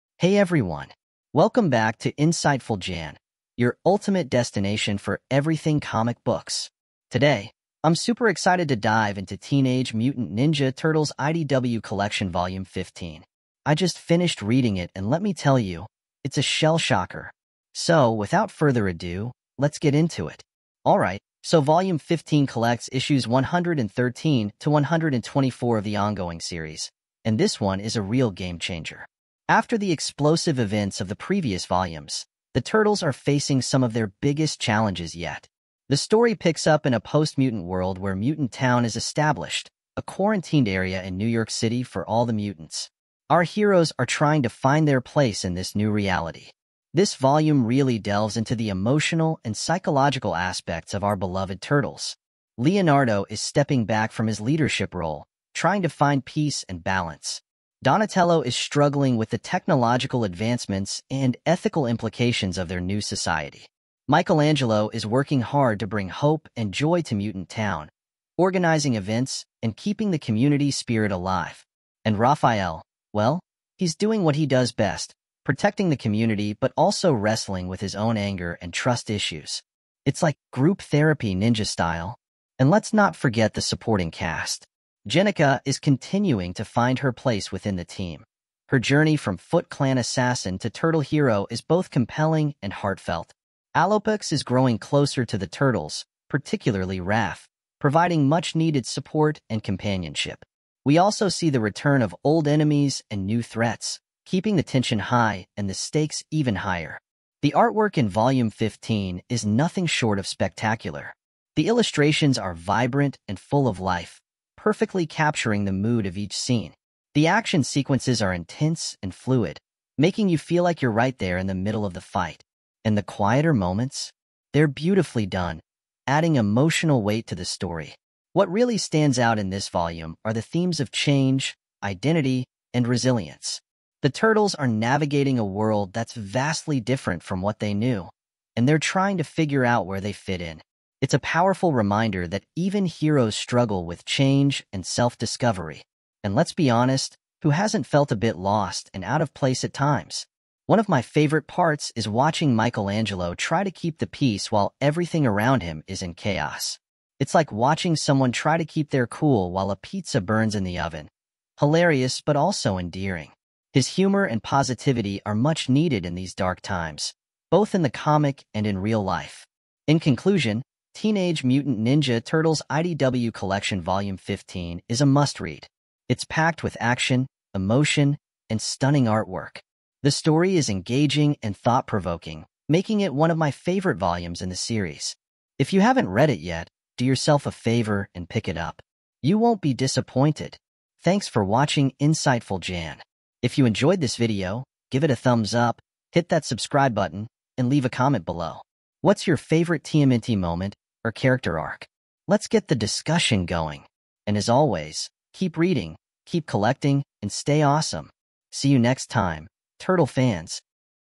Behind the bandana: TMNT Volume 15 IDW Deep Dive Review (Teenage Mutant Ninja Turtles)
🖌 This volume is not just a collection of stories; it's a homage to the enduring saga of the Teenage Mutant Ninja Turtles. We've woven together nearly a decade of storytelling into a masterpiece that's both a celebration of the past and a bold leap into the future.

👊 If you're a die-hard TMNT fan or just love a gripping tale, this volume is non-negotiable for your collection. It's the culmination of years of storytelling excellence, so get your copy and join us as we dissect every panel, every line, and every emotion that makes this collection a treasure.

Let's share this journey of ninja mastery, family bonds, and the never-ending adventure that is the Teenage Mutant Ninja Turtles. Cowabunga, dudes and dudettes—let's get this pizza party started!

Ebay Store